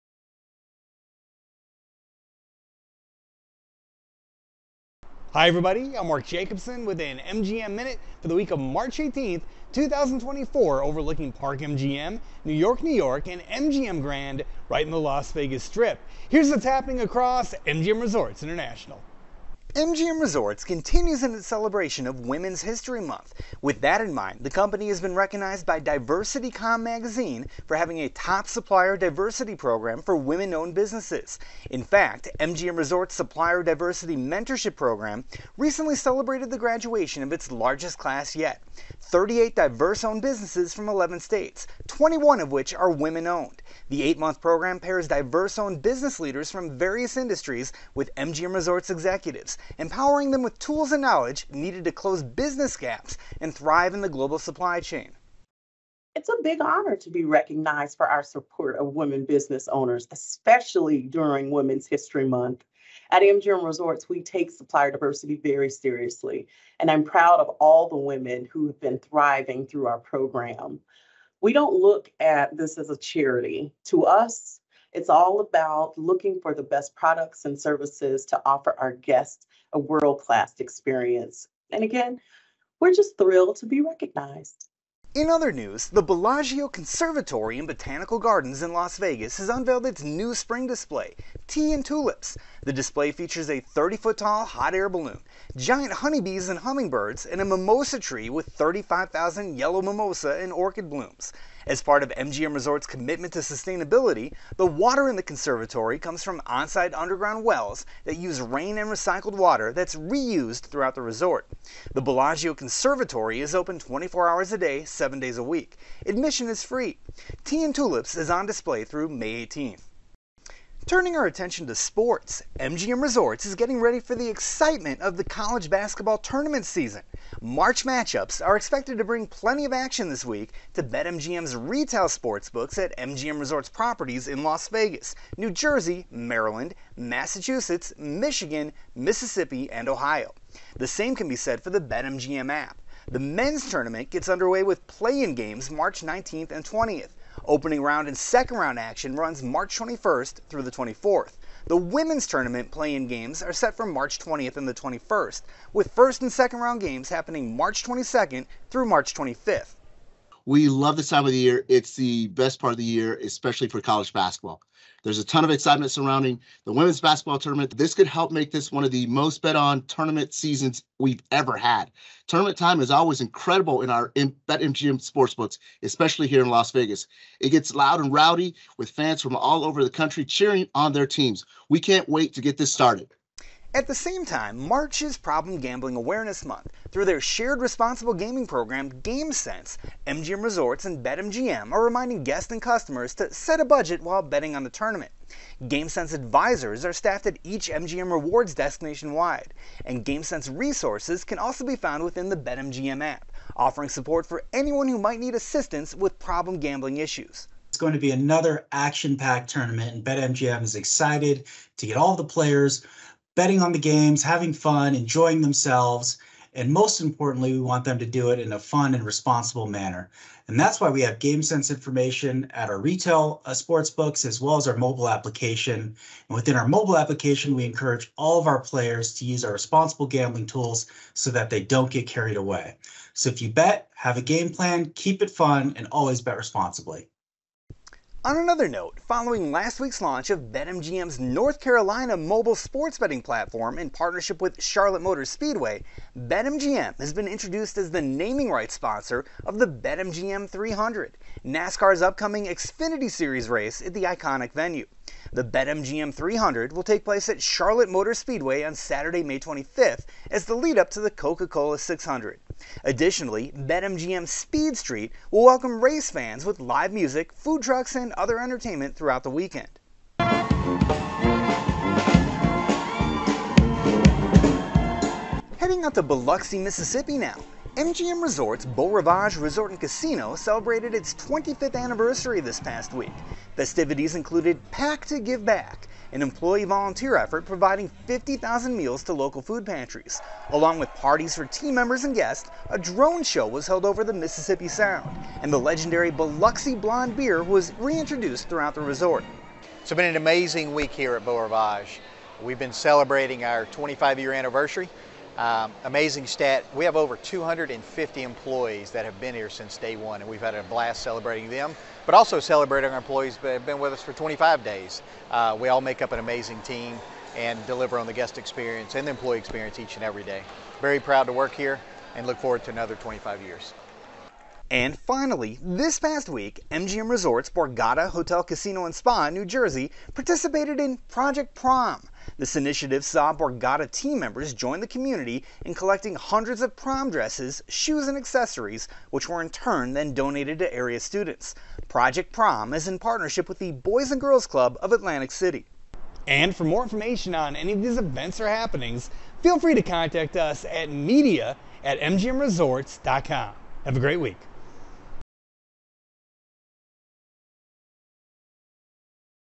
MGM Minute | March 18, 2024 | MGM Resorts
In this week’s MGMMinute: MGM Resorts recognized for support of women-owned businesses, Bellagio Conservatory unveils Spring display, March Matchups excitement returns to MGM Resorts’ BetMGM sportsbooks, Beau Rivage's 25th anniversary, and Borgata participates in Project Prom!